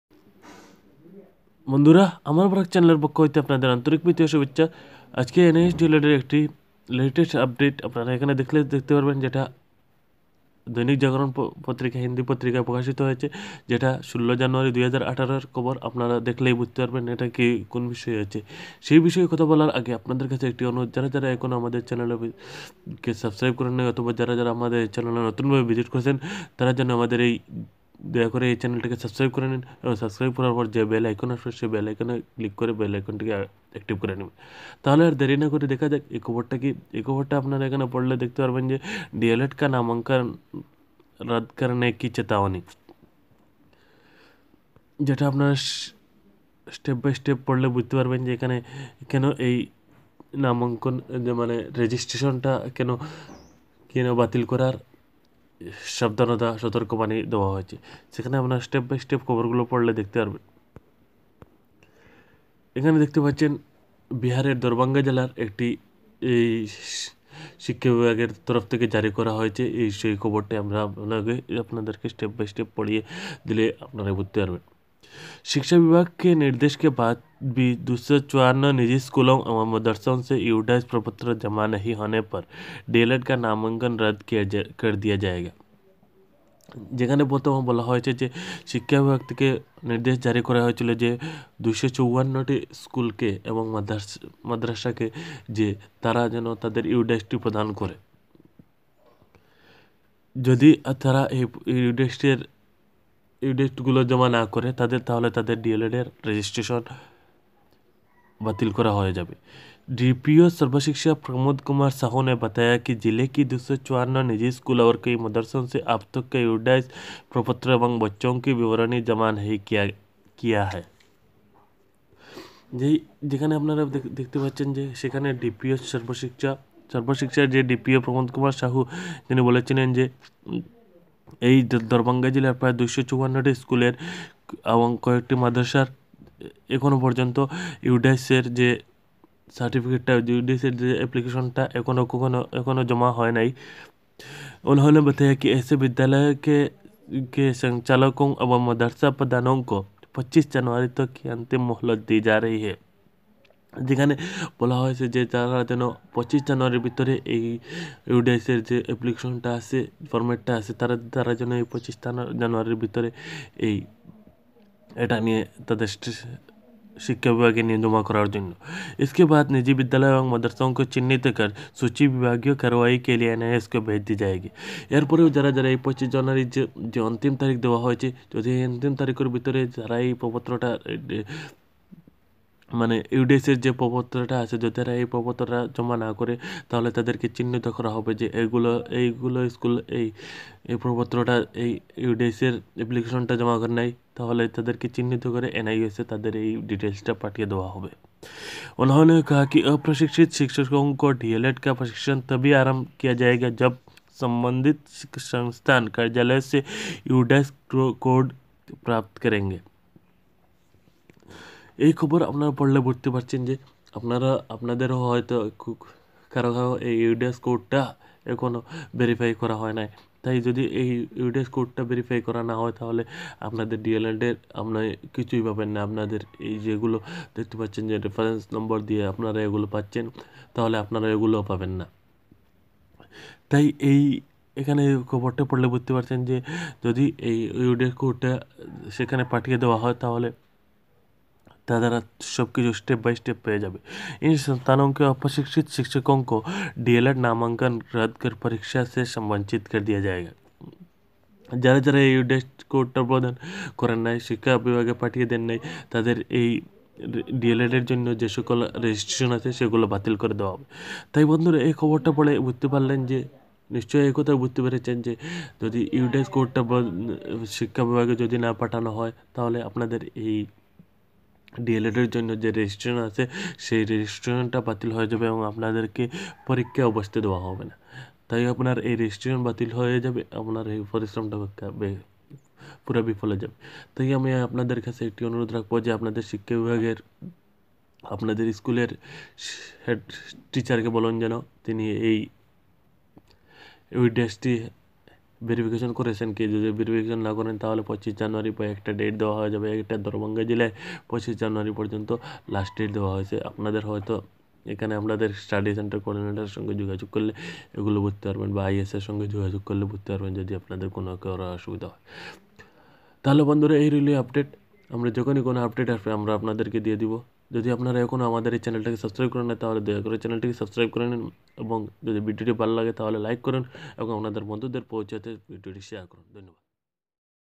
Nios D.el.ed নতুন খবর 16-1-18 রেজিস্ট্রেশন বাতিল হওয়ার সম্ভাবনা প্রবল ,যদি এই কাজটি হয় নাই
Nios deled registration